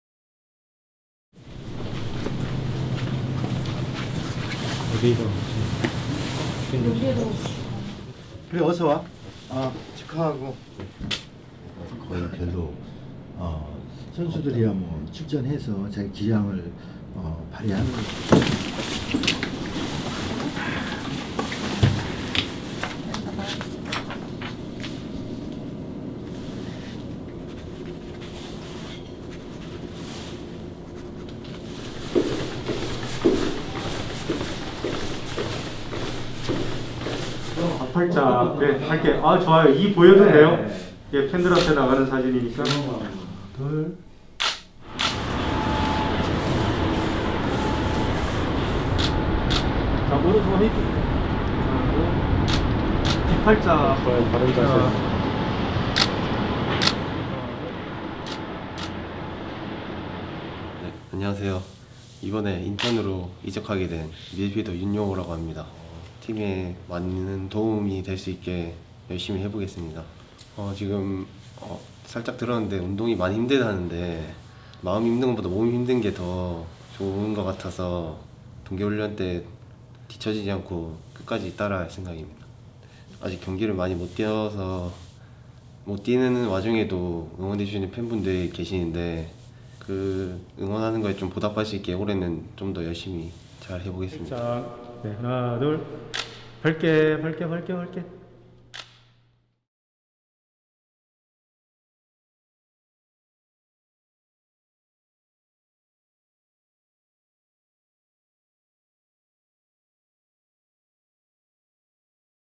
[IUFC TV] 인천 중원 강화해줄 테크니션 윤용호 영입 | 윤용호 오피셜 인터뷰
발기술이 뛰어난 ‘테크니션’ 미드필더 윤용호가 인천 유니폼을 입게 됐습니다.

윤용호 오피셜 현장 스케치 및 인터뷰 영상을 팬 여러분께 공개합니다.